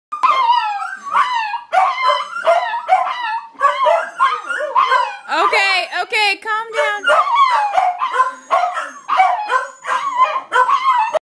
Corgis and Pig hate sirens
Sirens disturb the dogs